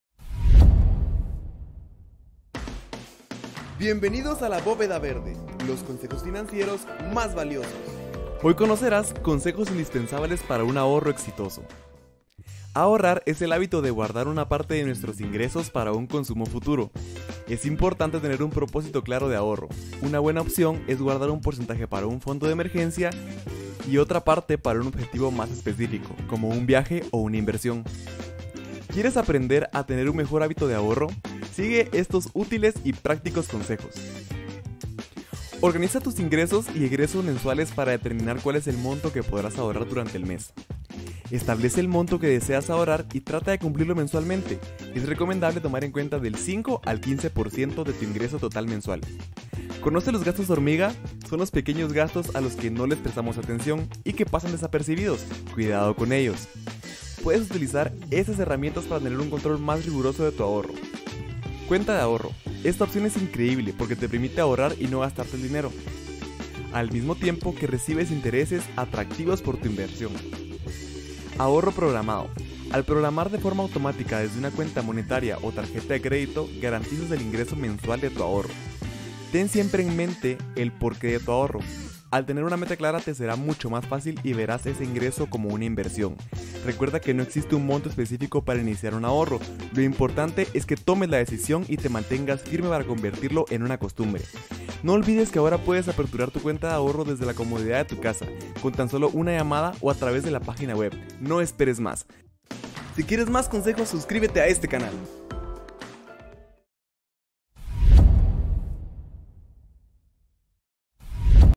¡Consejos indispensables para un ahorro exitoso! - La Bóveda Verde
¡Bienvenido a un episodio más de Bóveda Verde! 🙌 ¿Ya sabes cuál es tu propósito de ahorro? 🤔 Sigue los útiles consejos del video y mira como tus ahorros crecen, mes a mes.